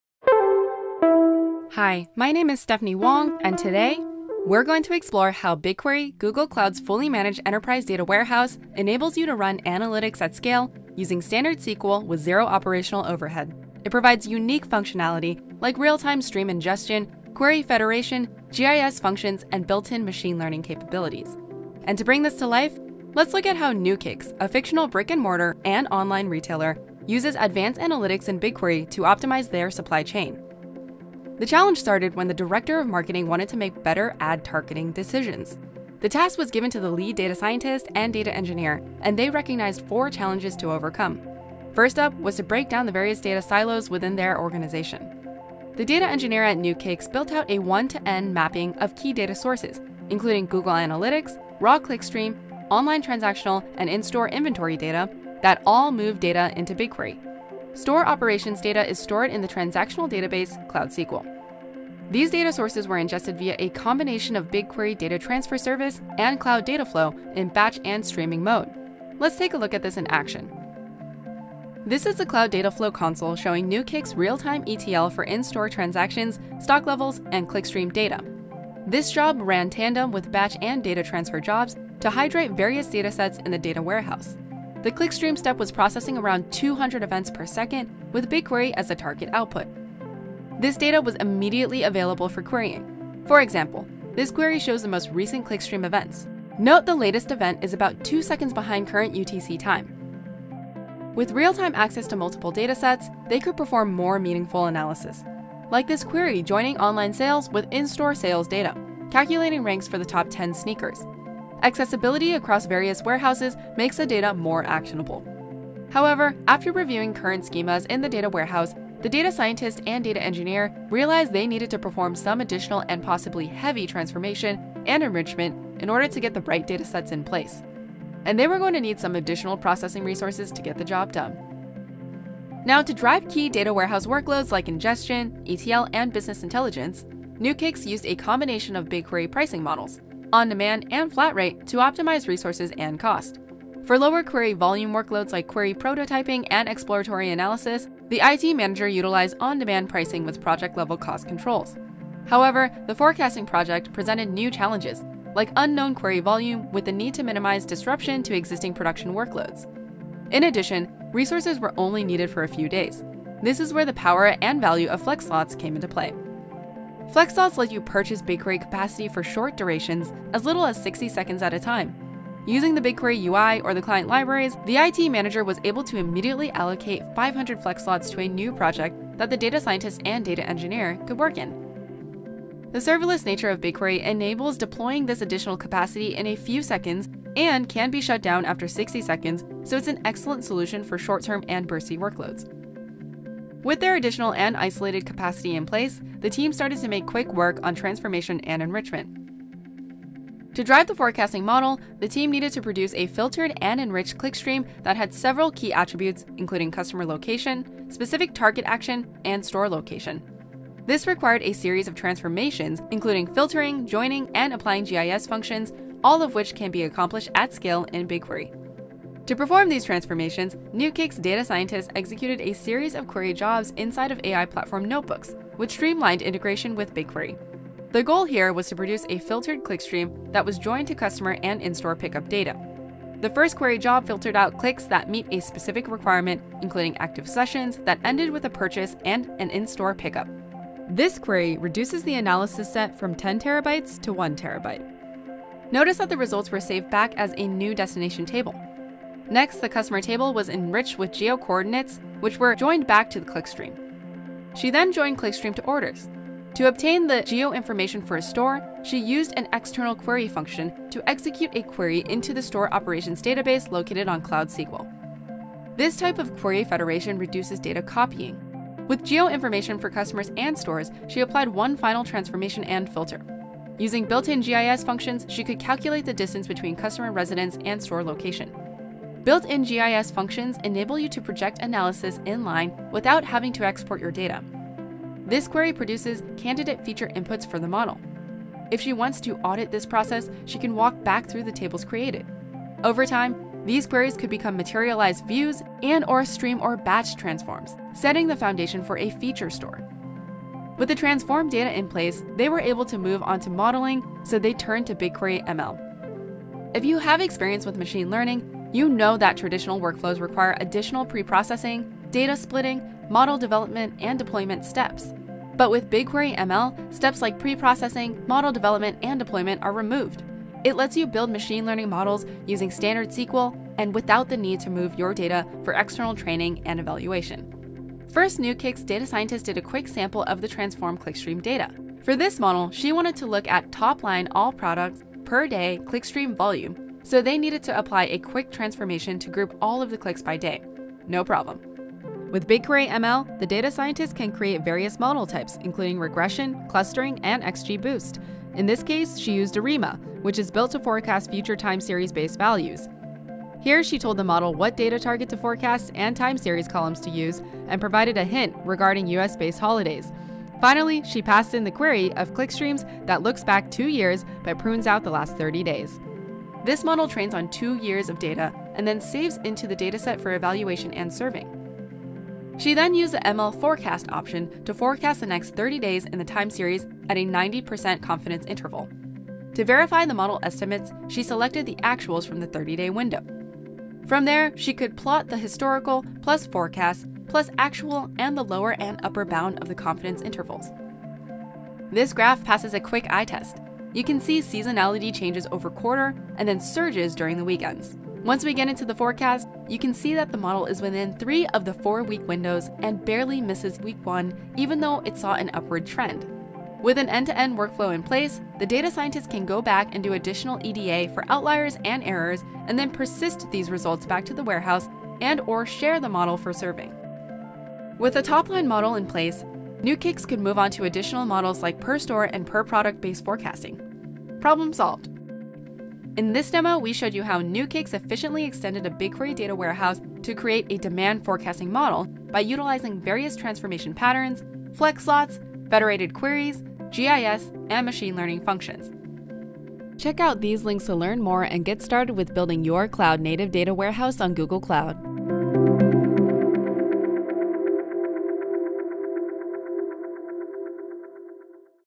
Modernize your data warehouse with BigQuery
In this video, we’ll show you how a brick & mortar and online retailer, uses advanced analytics in BigQuery to better forecast demand and optimize their operations in real-time. The demo showcases various transformation patterns, discusses BigQuery pricing models, including Flex Slots, and highlights federated queries, BigQuery GIS and BigQuery ML to help the organization make data-driven decisions.

Product: BigQuery; fullname: Stephanie Wong;